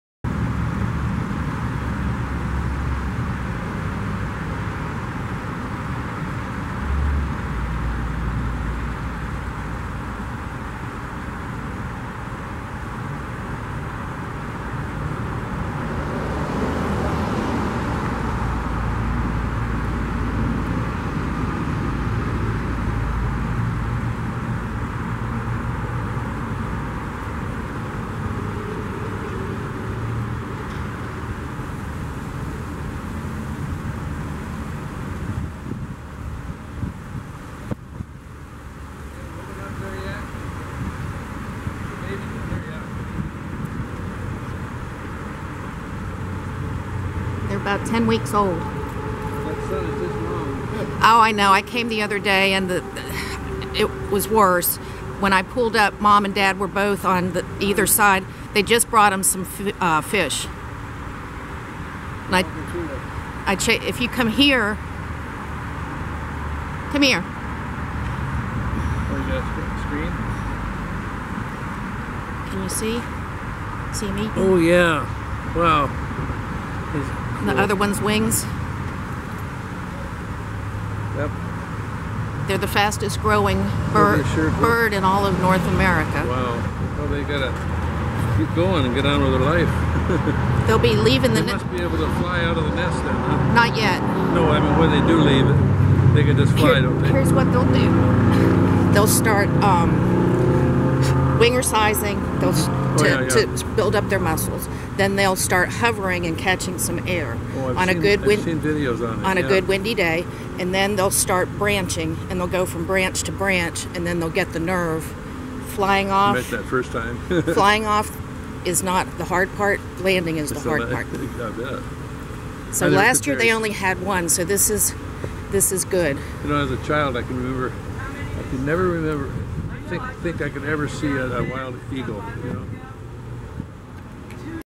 Dunne and Lawton
Juvies in the medical center parking lot on a gorgeous morning